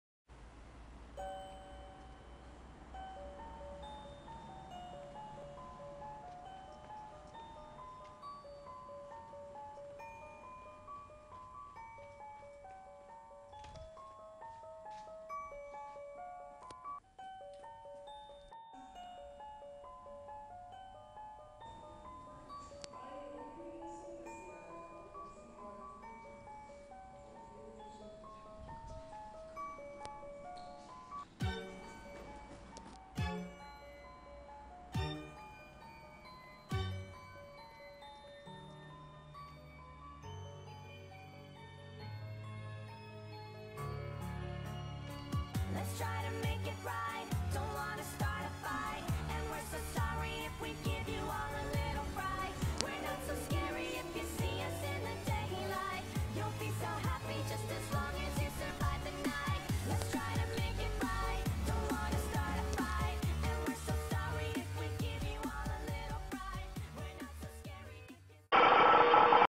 Unheimliche verlassene Münchner U Bahn Station
!!!ACHTUNG!!! ENTHÄLT JUMPSCARE AM SCHLUSS!!!